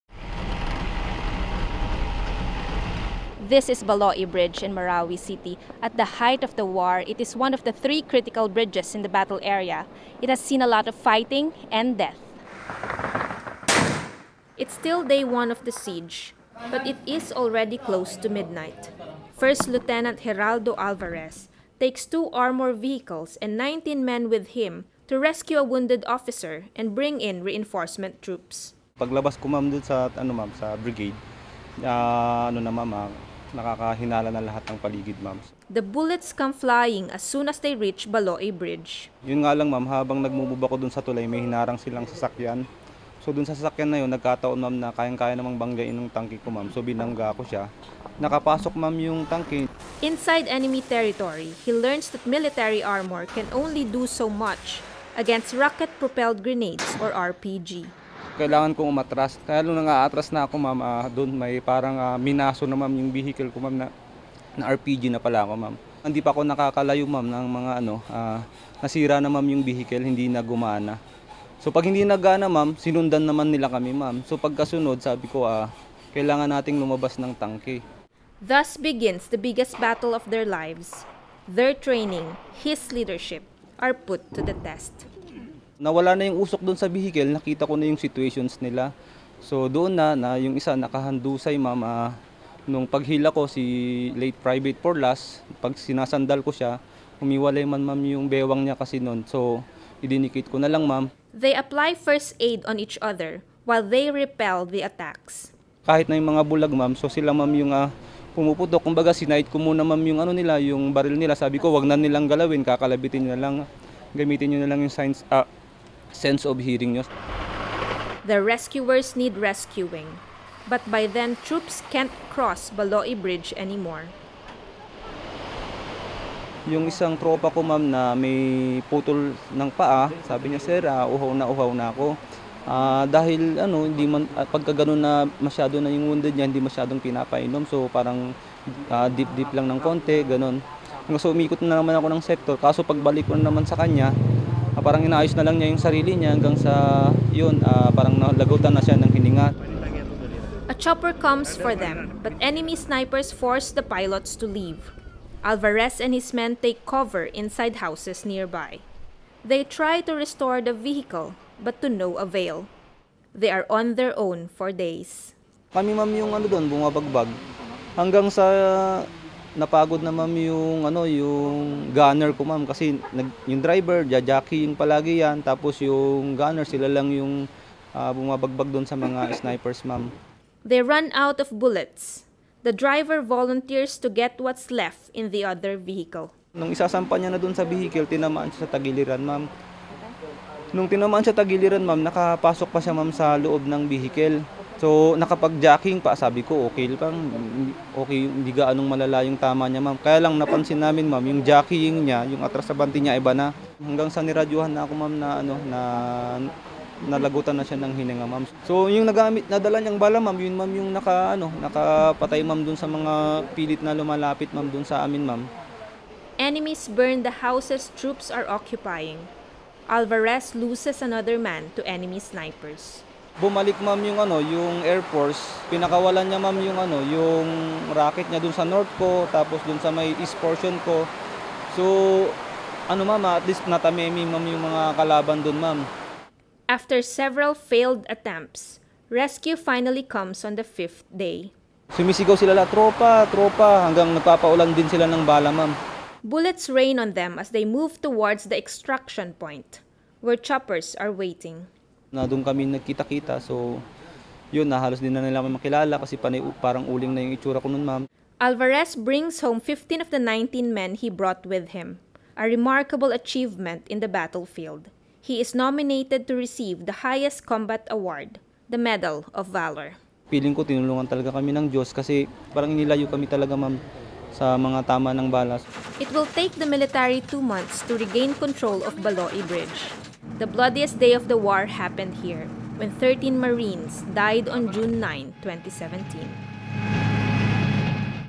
Why a Marawi officer is nominated for the highest combat award
More videos on Rappler Follow Rappler for the latest news in the Philippines and across the globe. First Lieutenant Geraldo Alvarez saves 15 of his men from certain death. He is nominated to receive the Medal for Valor, the highest and most rare combat award. FULL STORY

This video is part of Rappler's 35-minute documentary on Marawi.